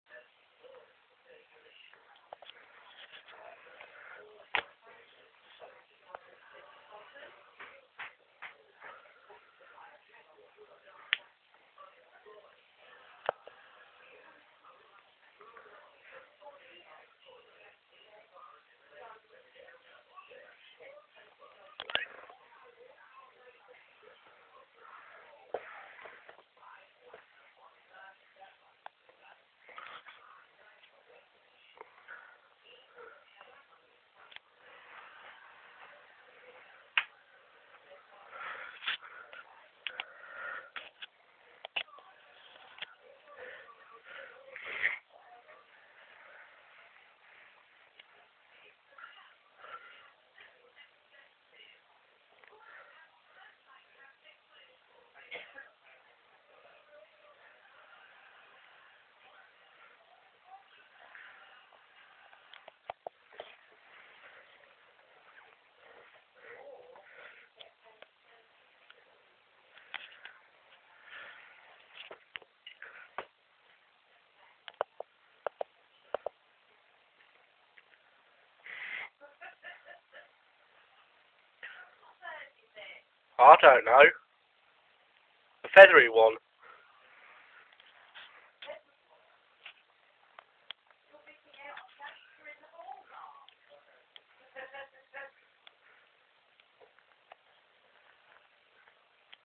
Cat plays with dead bird
Scarlet brought this bird in, too late to save it, we decided it was a bird of the feathery variety.